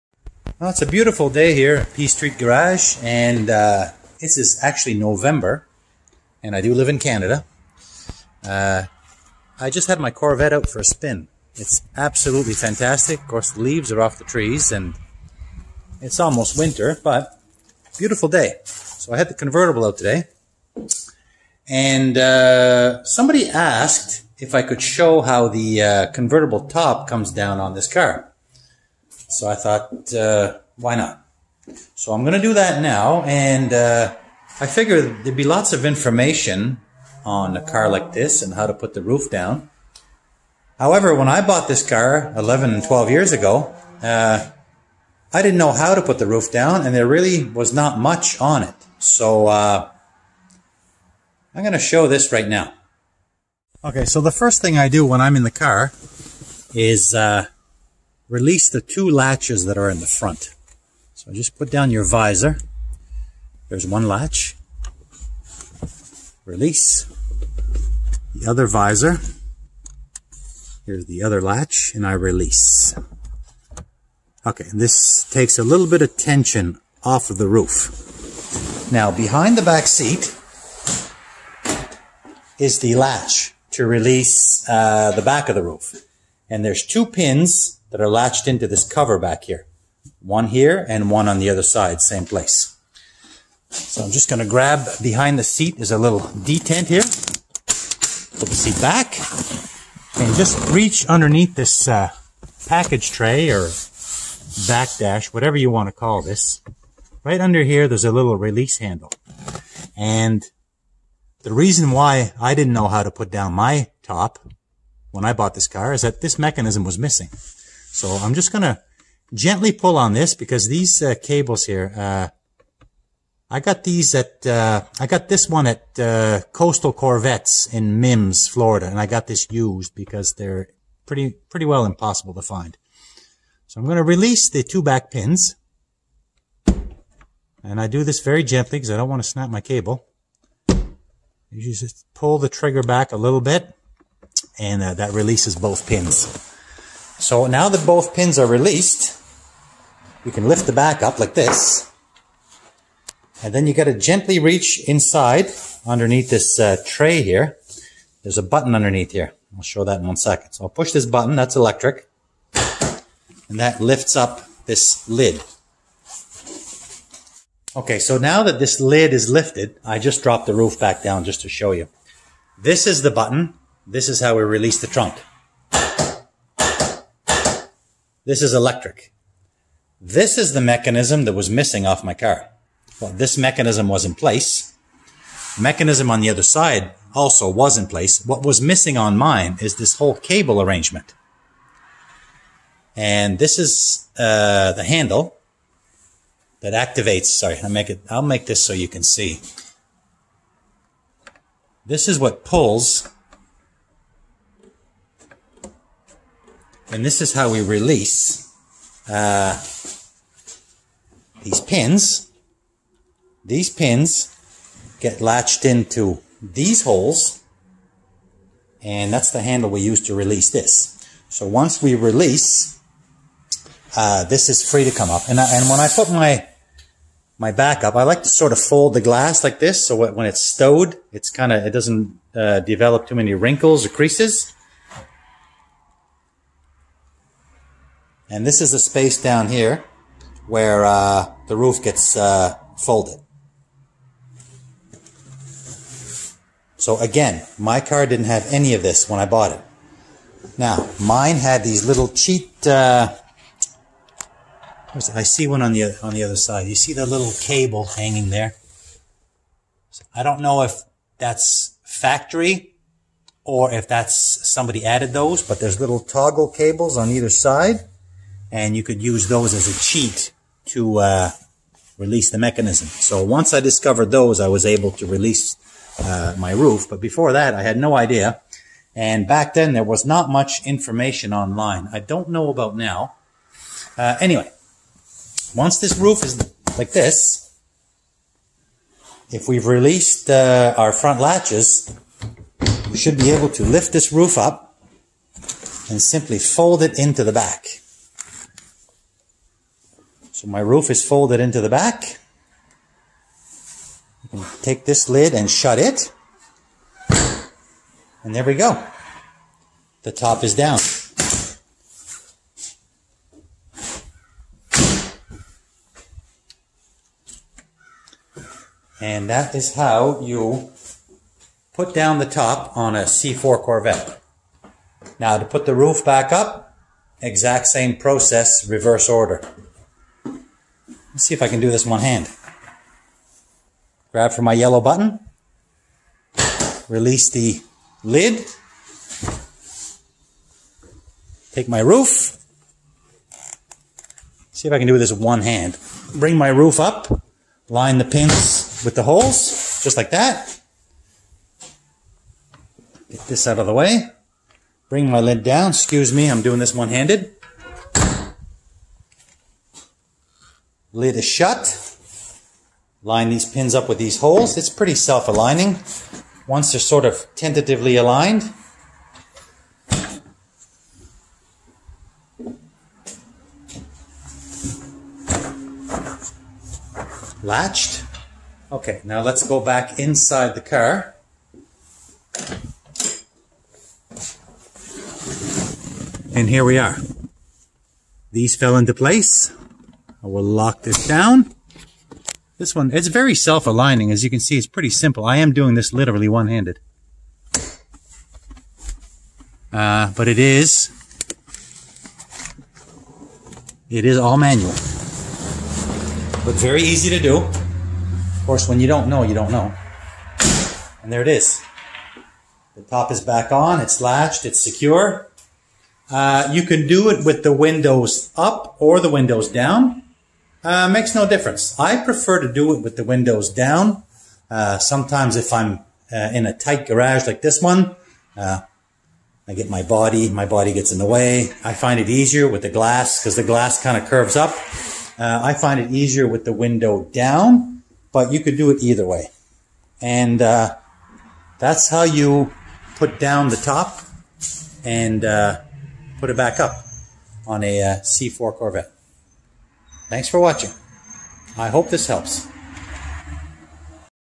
Convertible C4 Corvette. How to go topless.
C4 Corvette.  How to put the top down.
I just had my C4 Corvette out for a spin.  Although its November, I decided to go topless.
Someone asked to see how this was done so I decided to do a short video on the rag top and how it operates.
Its a fairly simple convertible top.
Its a manual top.